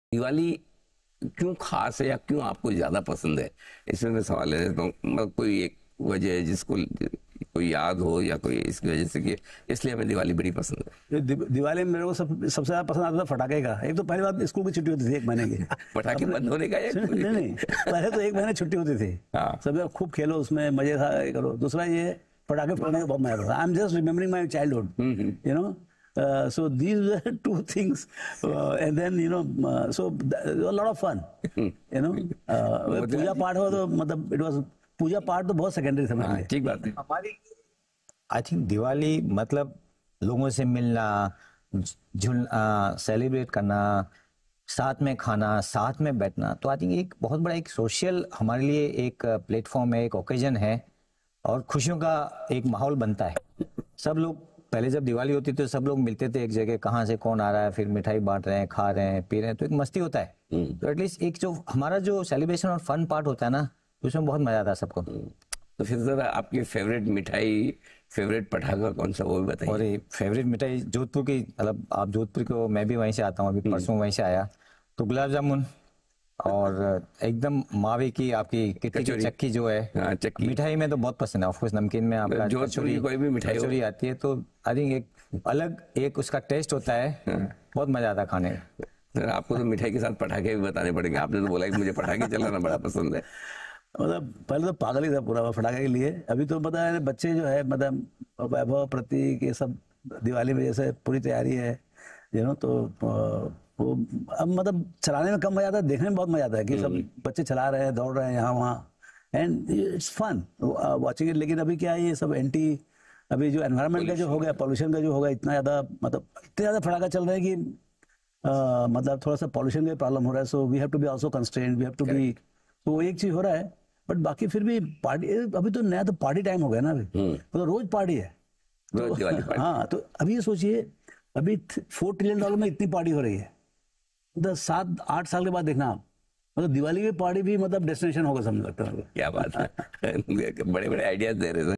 Raamdeo Agrawal And Motilal Oswal Remembered Their Old Memories with Anil Singhvi
Raamdeo Agrawal और Motilal Oswal को दिवाली क्यों है सबसे ज्यादा पसंद? Raamdeo Agrawal और Motilal Oswal की फेवरेट मिठाई कौन सी है? Raamdeo Agrawal और Motilal Oswal ने ताजा की पुरानी यादें... 'खुशियों वाली दिवाली' में मार्केट के महारथी रामदेव अग्रवाल और मोतीलाल ओसवाल से Anil Singhvi की बातचीत...